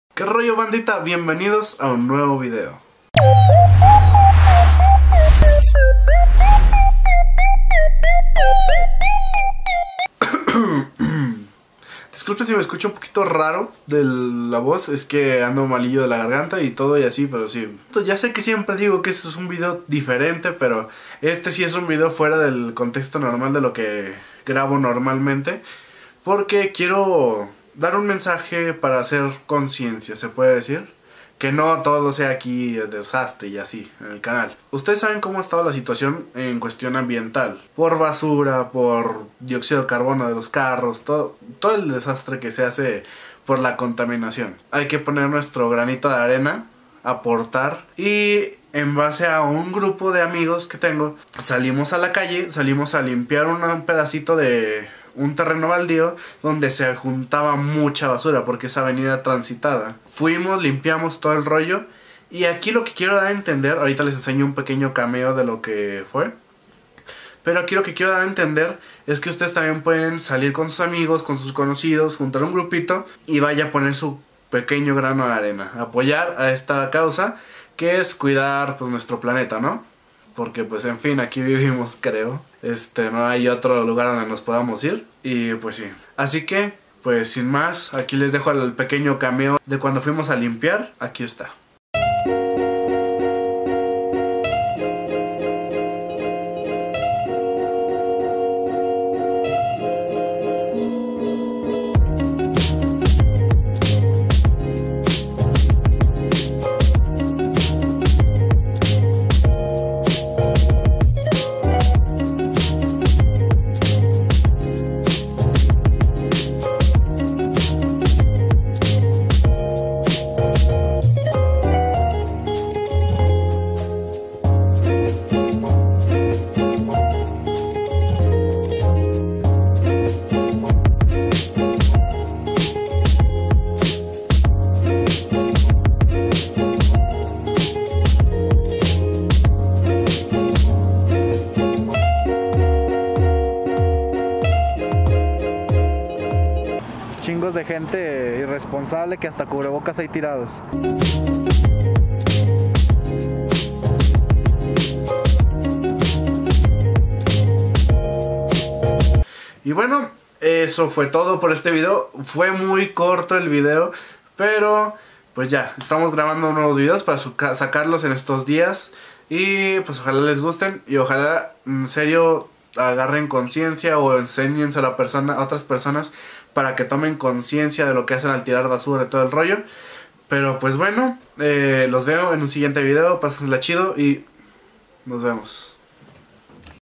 Pon de tu parte | Limpiando la calle
Bandita esta ves hicimos algo que llevaba mucho tiempo queriendo hacer, salí con un grupo de amigos a limpiar una zona con mucha basura de mi ciudad, espero con este video se animen a hacer el cambio ustedes también. :D